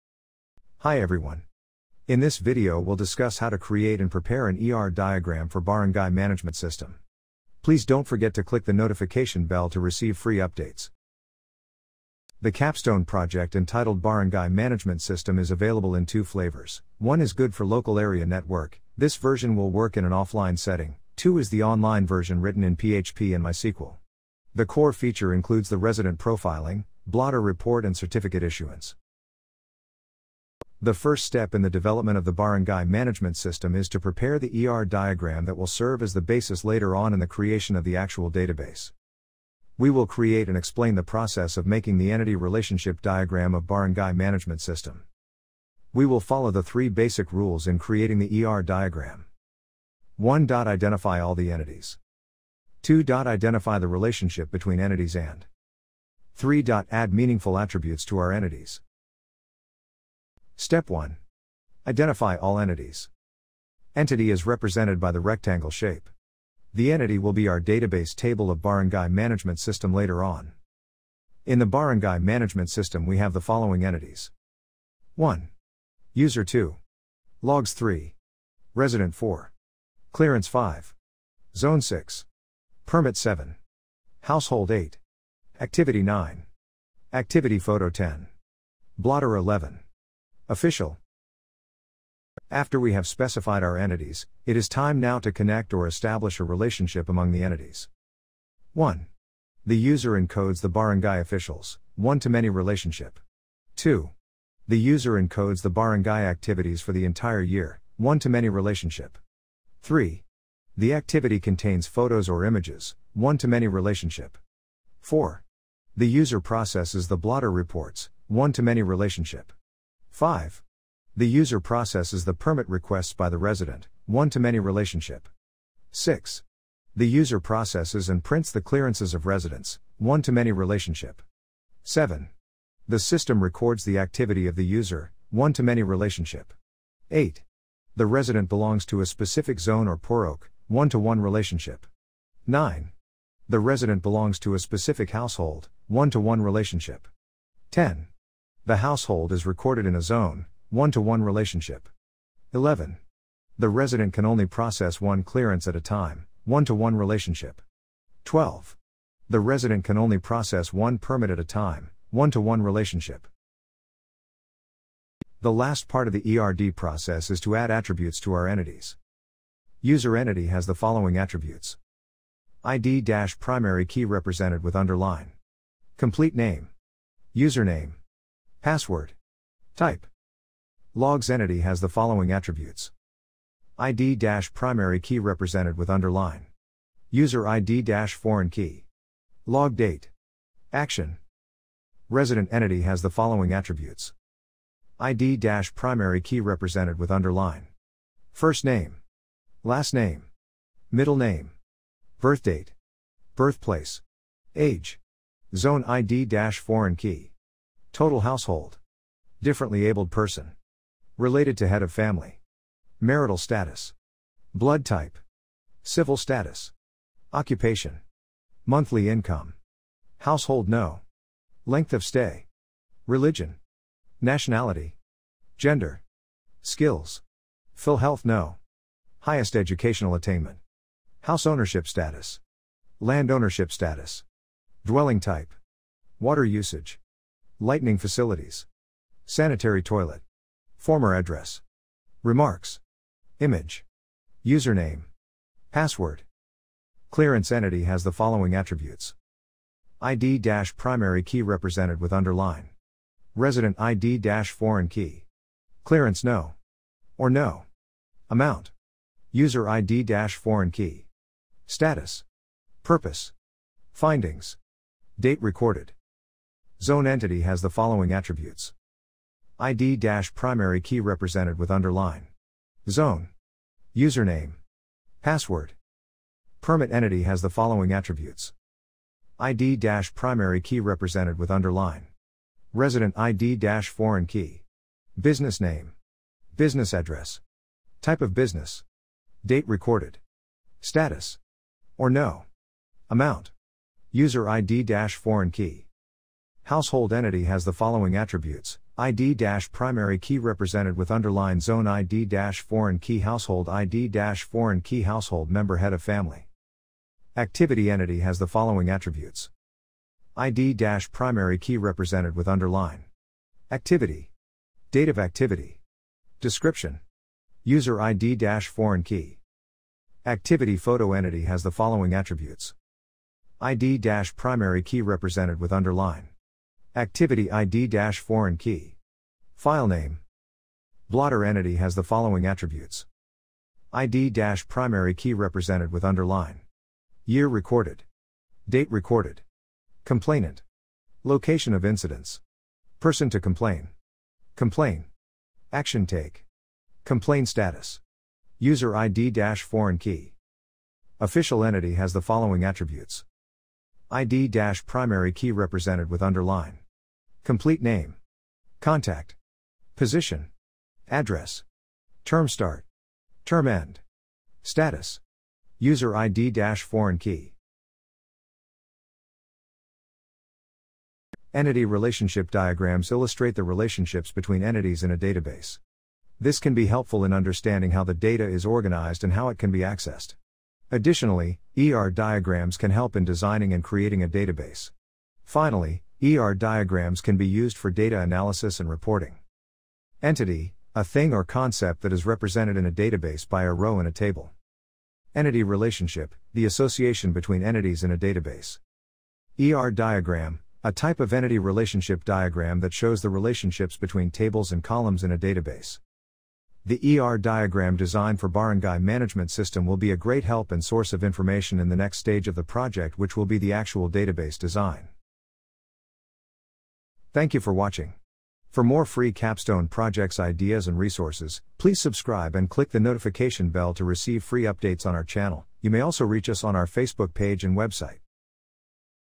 Barangay Management System Entity Relationship Diagram
Barangay Management System is a tool to help Barangay Officials manage information about their constituents. It is a desktop application that contains basic information about every resident of the barangay. It also has the capability to generate reports for the different categories of information. The system also has the capability to manage barangay clearances.

An Entity Relationship Diagram (ERD) is a graphical representation of the relationships between entities in a database. ERDs can be used to design databases, or to test whether an existing database is designed optimally. ERDs are a crucial tool for any database designer, as they provide a visual representation of the database structure. This makes it easier to identify and correct inconsistencies in the database. Additionally, ERDs can be used to determine which entities are related and how they are related. This information can then be used to create tables and views in the database, or to create relationships between tables. Additionally, ERD can help you understand how data is stored within the database, which can help you to optimize the database layout. Consequently, ERDs are essential for any database designer, and should be included in every database design project.